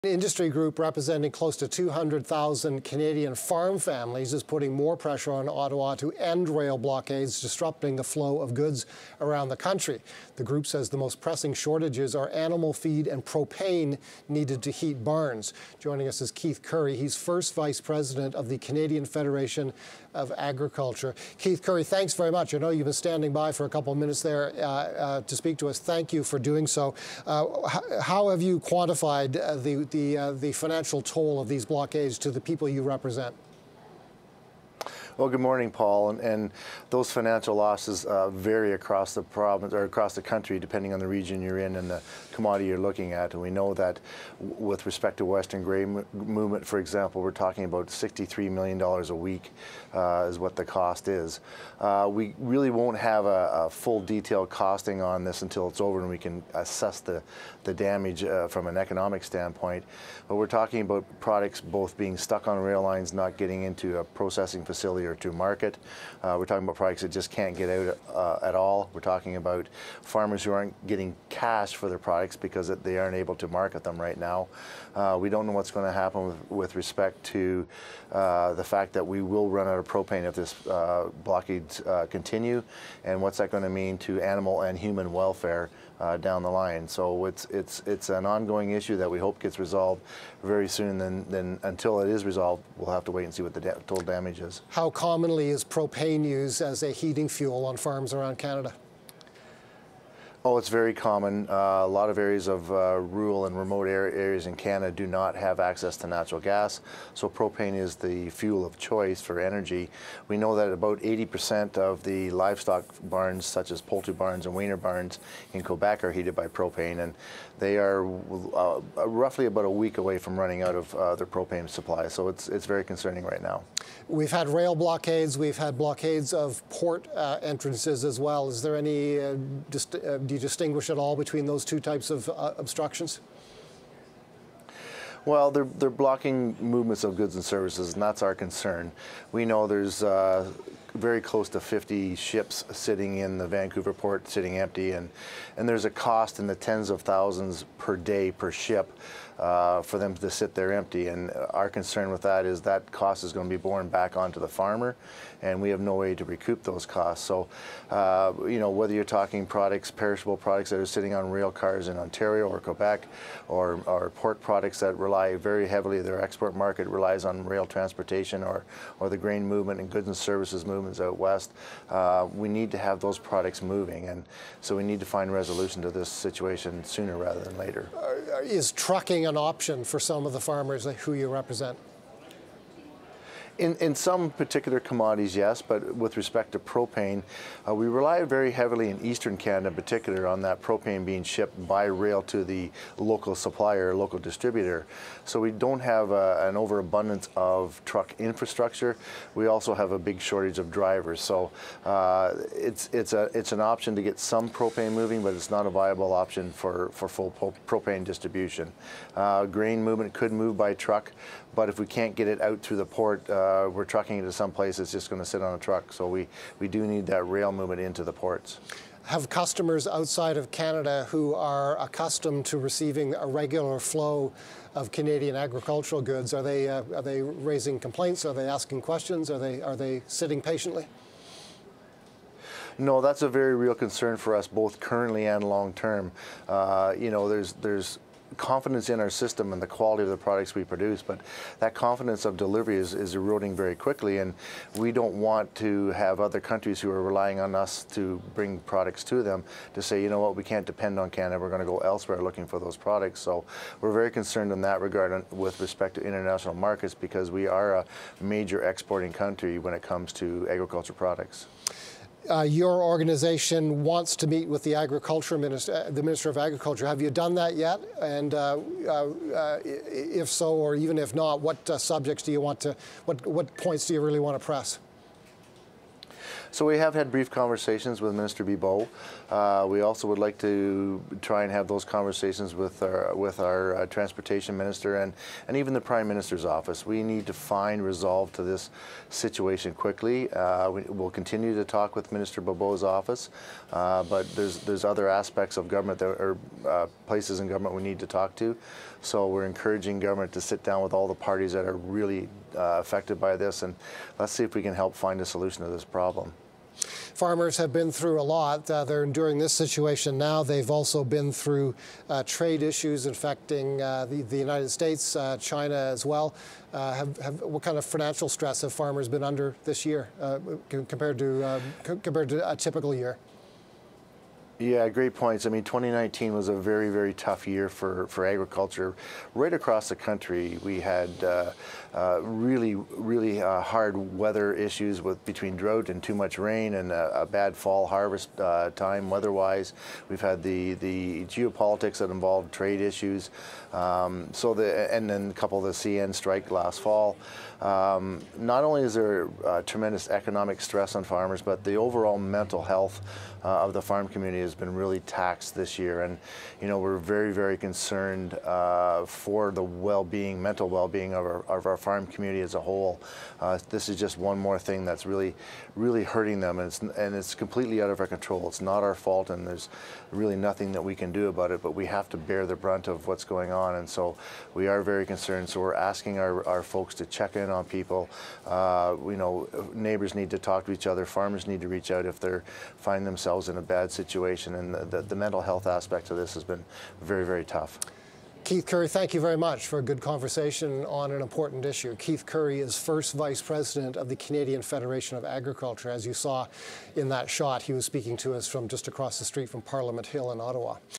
Rail blockades costing farmers $63M per week: Canadian Federation of Agriculture
Keith Currie, first vice president, Canadian Federation of Agriculture discusses the impact of the rail blockades on farmers' incomes. Delays in propane shipments are a pressing concern for farmers who need to heat barns for livestock.